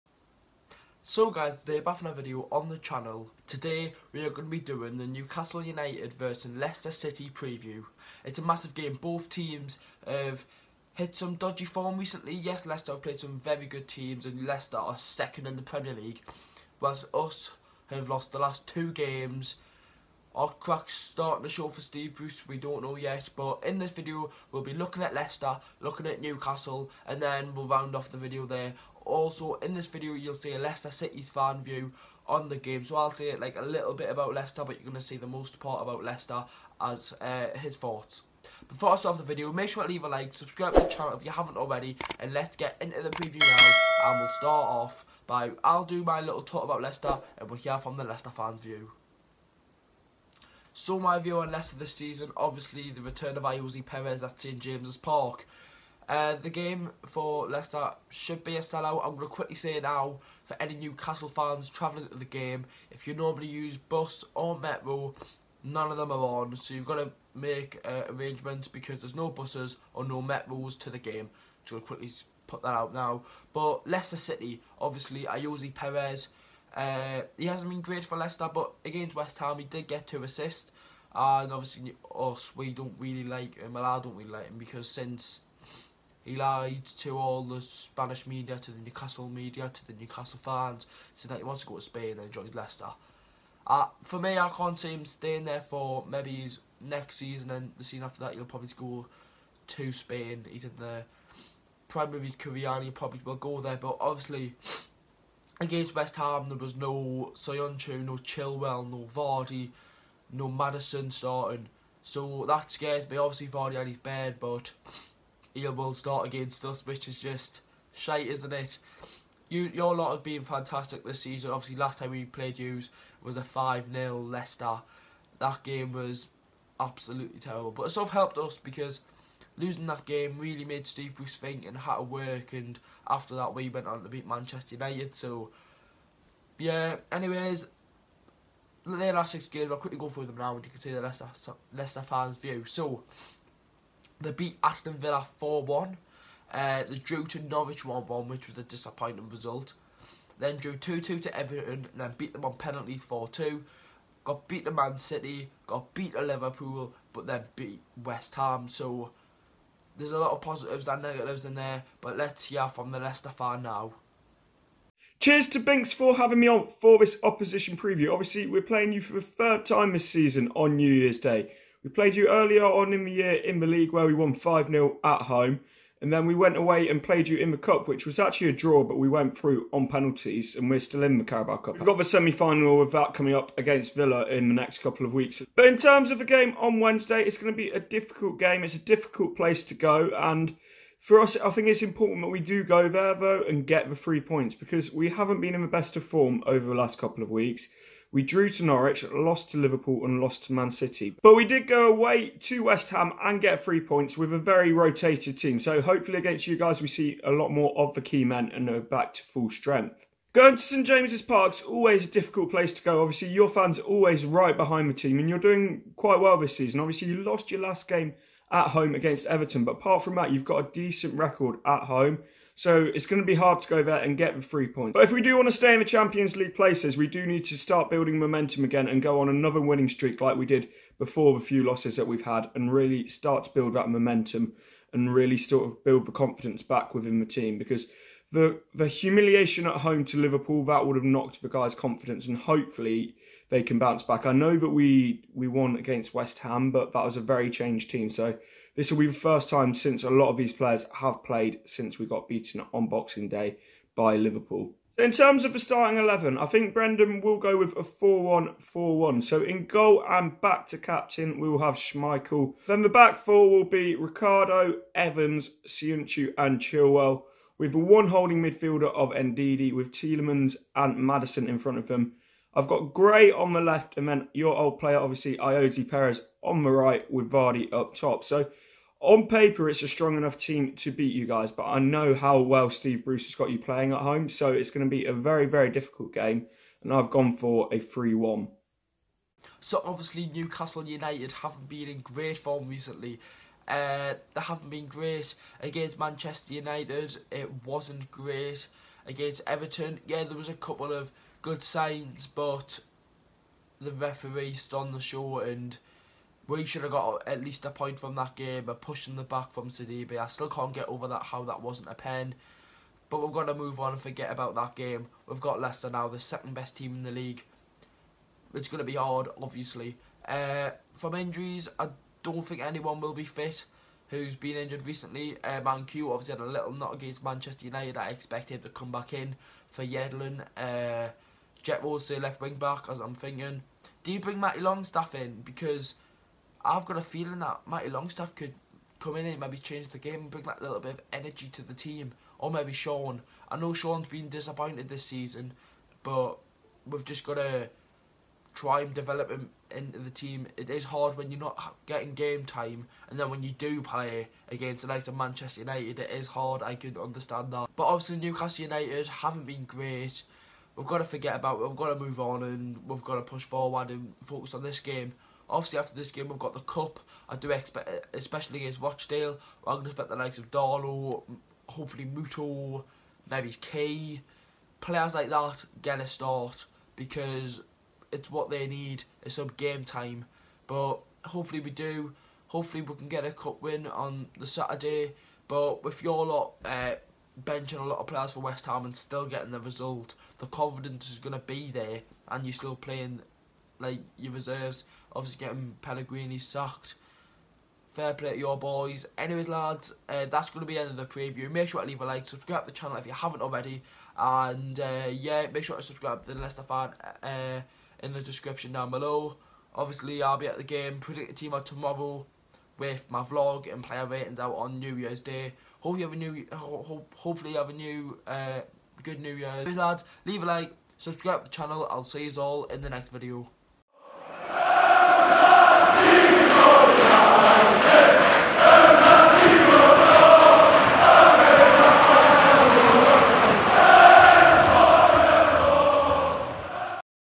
*PEREZ RETURNS AGAIN* NEWCASTLE VS LEICESTER PREVIEW! LCFC BOYA VIEW!
Big game on New years day vs a fantastic Leicester team!